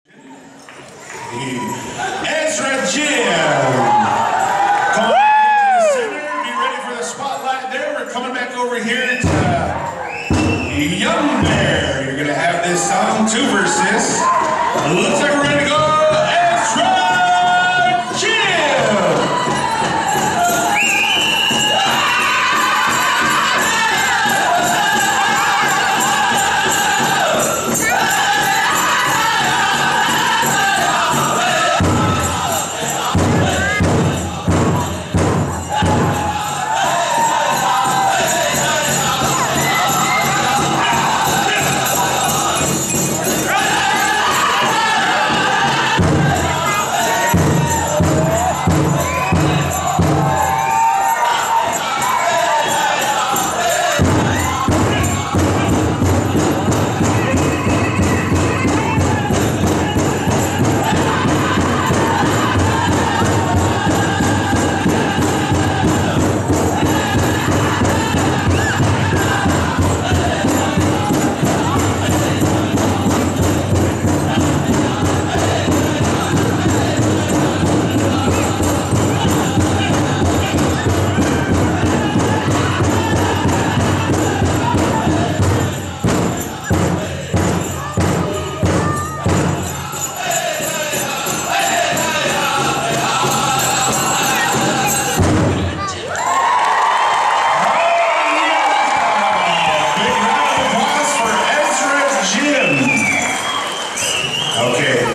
Ezra Jim @ Shelton Powwow 2017 (Spotlight Solo)
Peyton Whitebuffalo grass dance specail
Little Creek casino Powwow
Shelton,WA